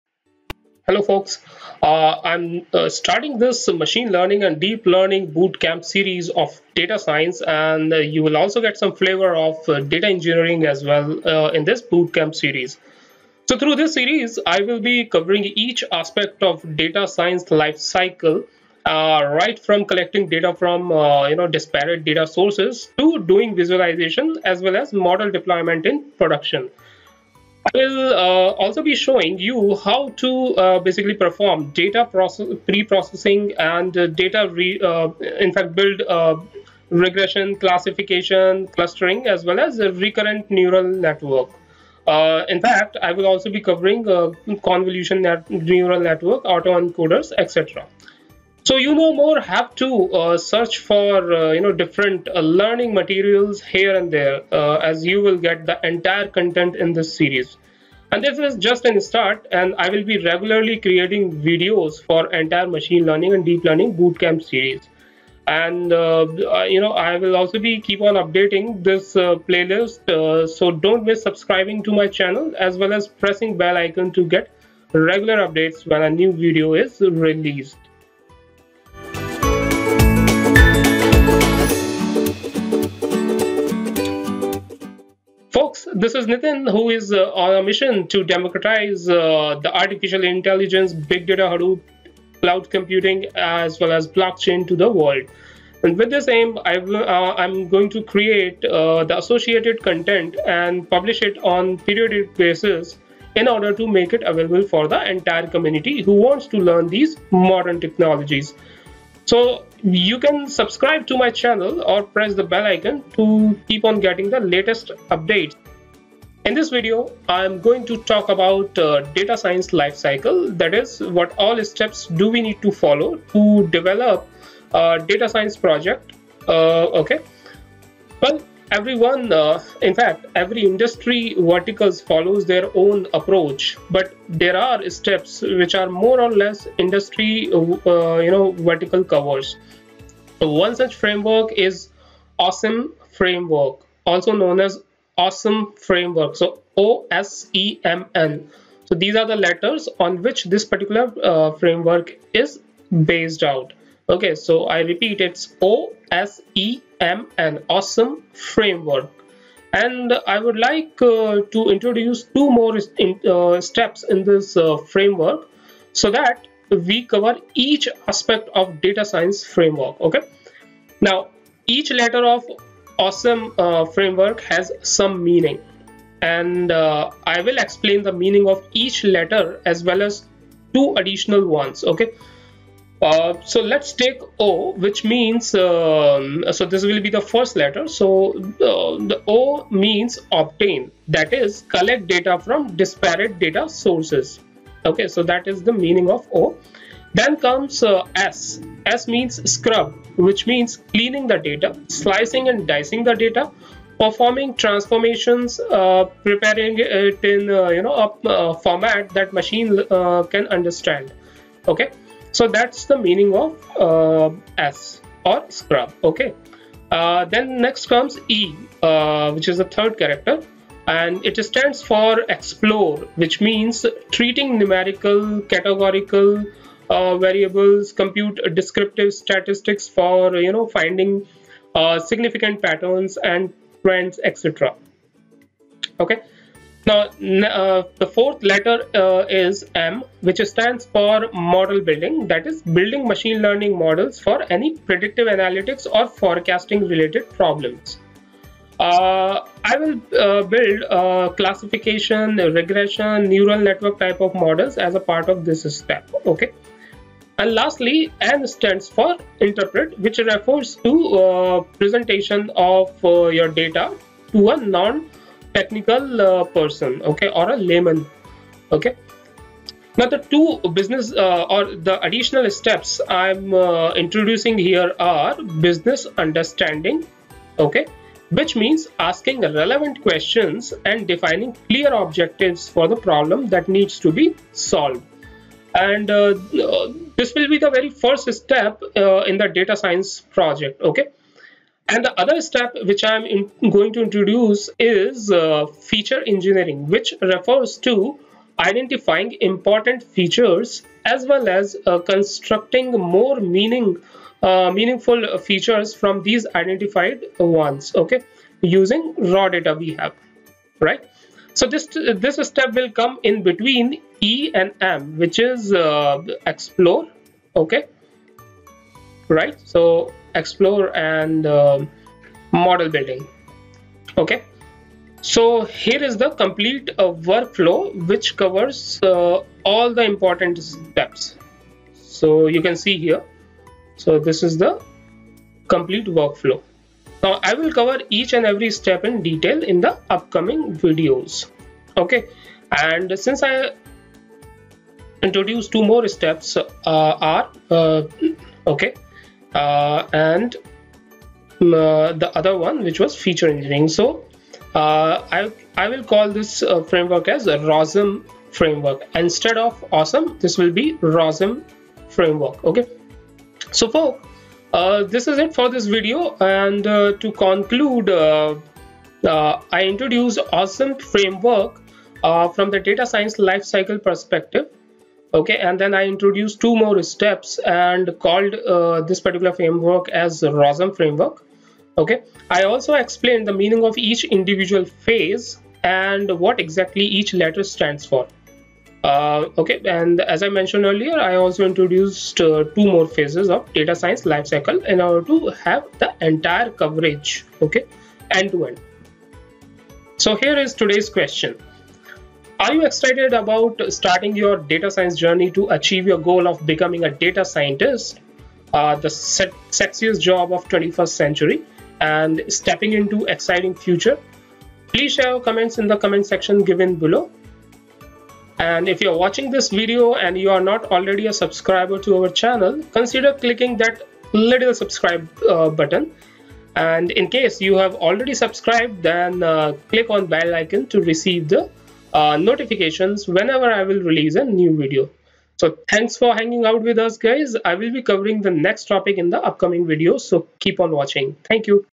Free Data Science Course Online - OSEMN Framework | Introduction
Free Data Science Course Online - OSEMN Framework
This is a machine learning & deep learning Bootcamp series of data science. You will also get some flavor of data engineering as well in this Bootcamp series. Through this series, you will be able to learn each aspect of the Data science lifecycle right from collecting data from disparate data sources, data preprocessing to doing visualization as well as model deployment in production. You will also see how to perform data preprocessing and build, regression, classification, clustering as well as a recurrent neural network, convolution neural network, autoencoders, etc. Through this series, you will be able to learn everything pertaining to Machine and Deep Learning in one place. Content & Playlist will be updated regularly to add videos with new topics.

Subtitles available in: Hindi, English and French